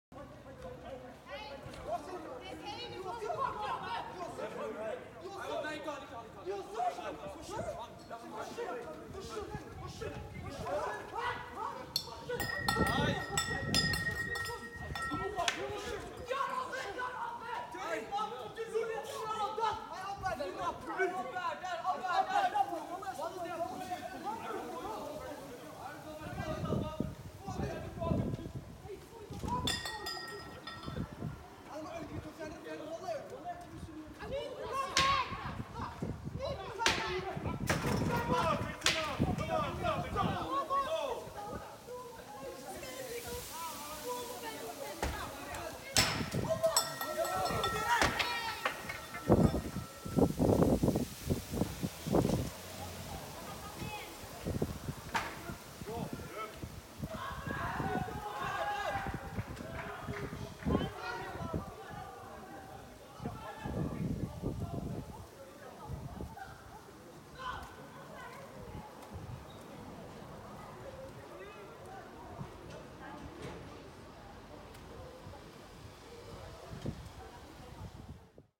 Her går guttegjengen til angrep på utested i Oslo
OSLO: Videoen, som er filmet onsdag, viser en gjeng unge gutter gå løs på puben Bydels Kro på Tøyen i Oslo. Video: Privat